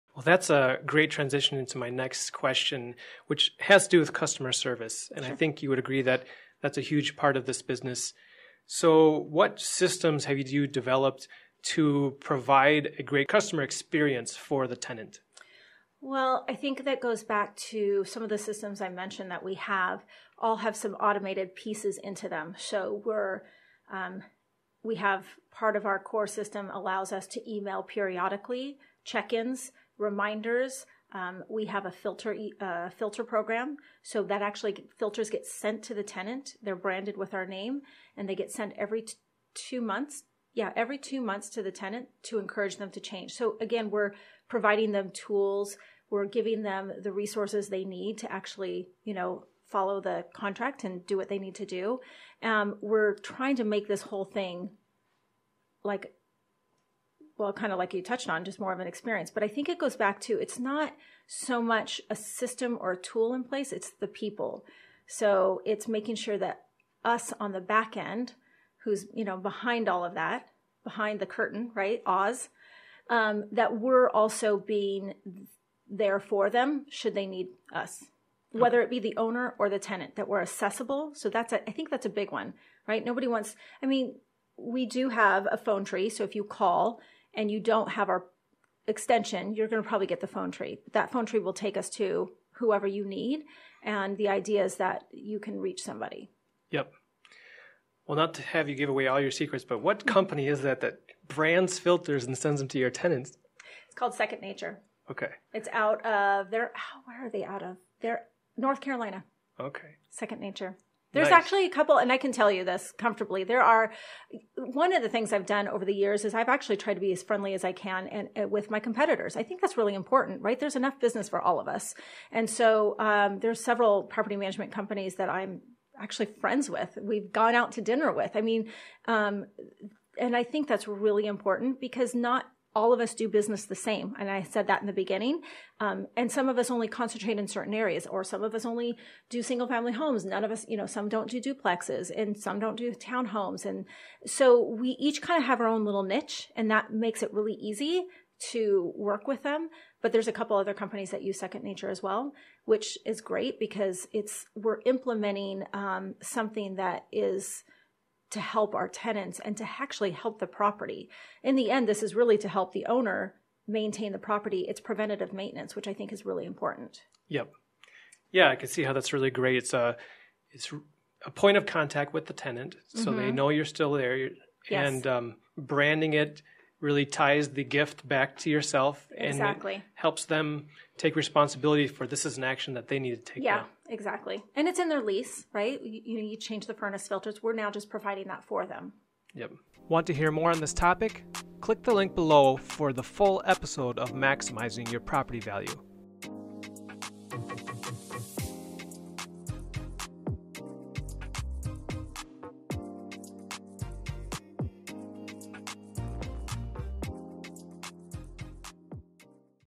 Creating A Great Customer Experience For Our Residents (MYPV E14.9)
Having automated systems will help you to create a better customer experience for your residents.

Beyond that it's important that you have the right people in place. The property management company should be accessible. It should be clear who is responsible for what and have clear expectations about getting back to people in a reasonable amount of time.

Check out the full episode and show notes of Maximizing Your Property Value